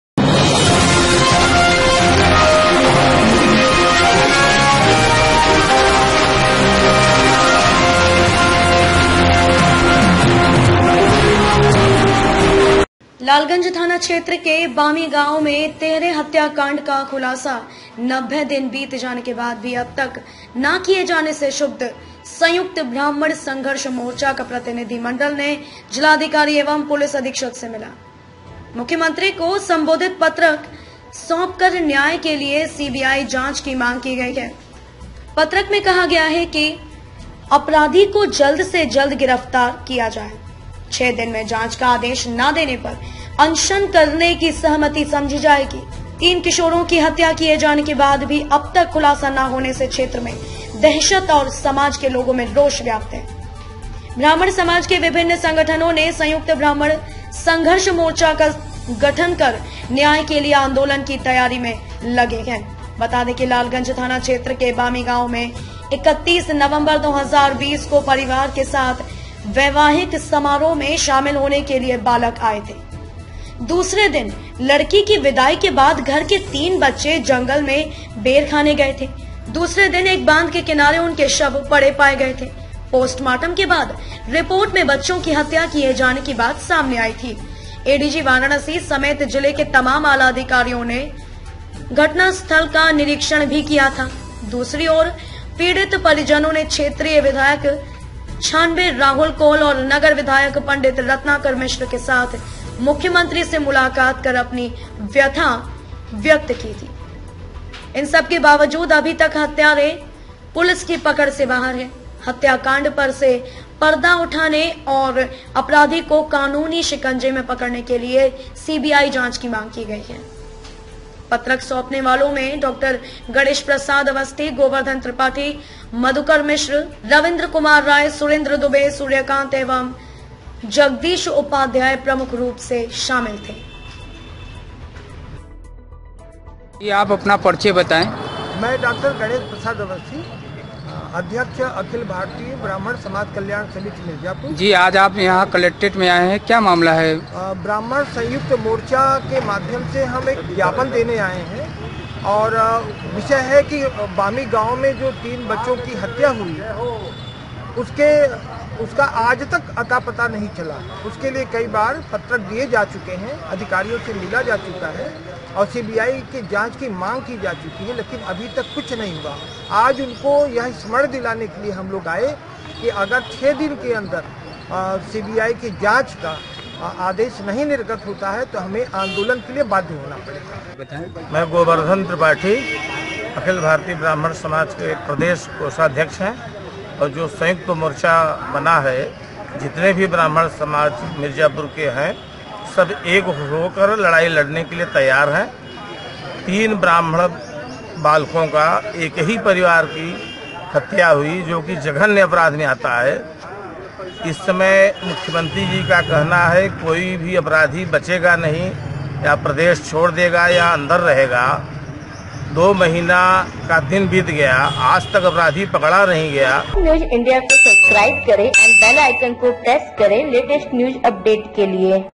Mirzapur:- बामी हत्याकांड से झुब्ध संयुक्त ब्राह्मण संघर्ष मोर्चा डीएम से मिला, सीबीआई जांच की मांग।
मीरजापुर । लालगंज थाना क्षेत्र के बामी गांव में तिहरे हत्याकांड का खुलासा 90 दिन बीत जाने के बाद भी अब तक न किए जाने से झुब्ध संयुक्त ब्राह्मण संघर्ष मोर्चा का प्रतिनिधि मंडल ने जिलाधिकारी एवं पुलिस अधीक्षक से मिला । मुख्यमंत्री को संबोधित पत्रक सौंपकर न्याय के लिए सीबीआई जांच की मांग की गई है । पत्रक में कहा गया है कि कहा कि अपराधी को जल्द से जल्द गिरफ्तार किया जाय । छह दिन में जांच का आदेश न देने पर अनशन करने की सहमति समझी जायेगी । तीन किशोरों की हत्या किए जाने के बाद भी अब तक खुलासा न होने से क्षेत्र में दहशत और समाज के लोगों में रोष व्याप्त है । ब्राह्मण समाज के विभिन्न संगठनों ने संयुक्त ब्राह्मण संघर्ष मोर्चा का गठन कर न्याय के लिए आंदोलन की तैयारी में लगे हैं ।